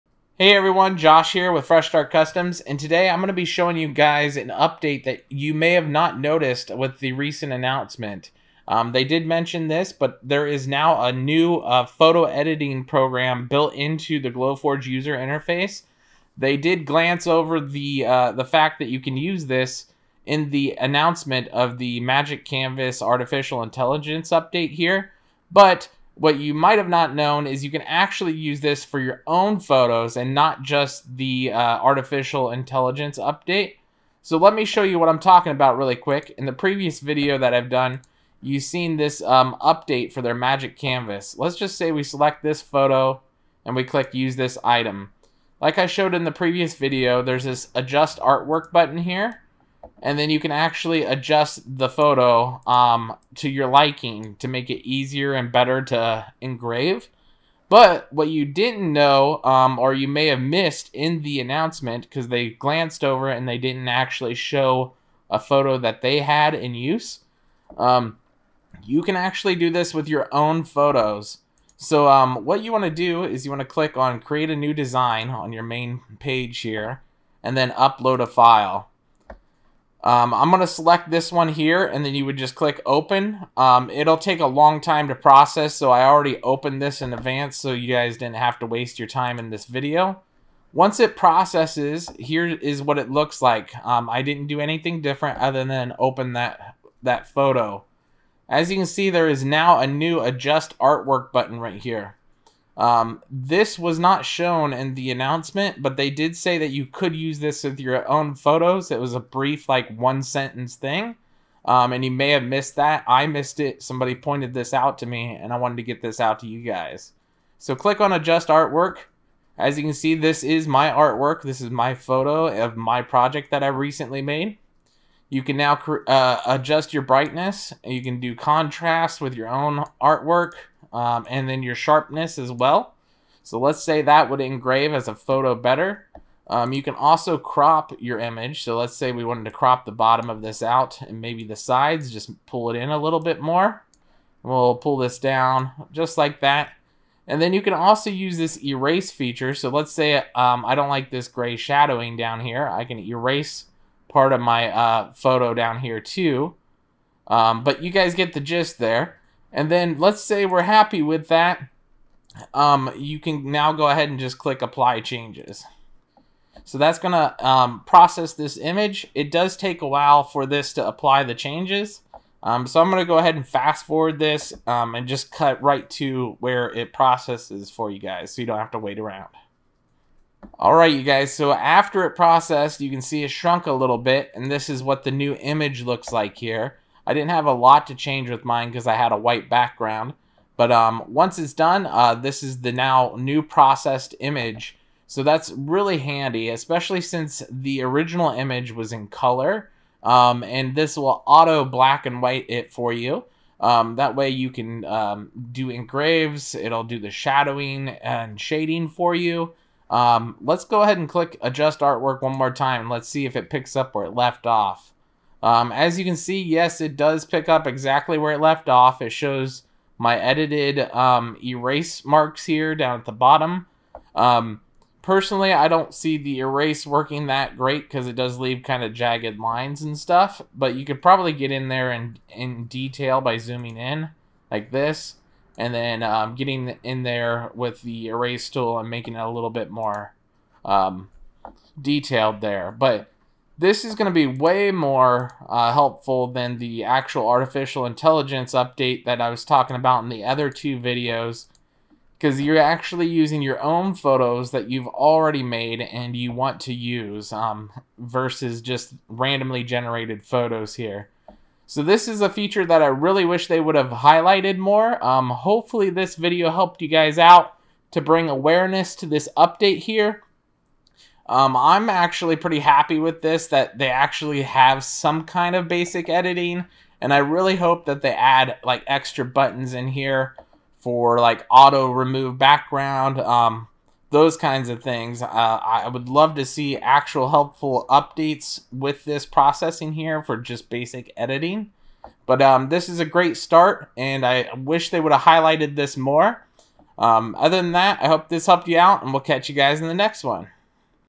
Edit your own photos in the Glowforge UI
This video is about the feature that was mentioned in passing in the latest announcement but was never showed with your own uploaded photos. This is way more useful the the AI photo editor and thought I should make a video featuring it to show you how to use it with your own photos.

then click on main list.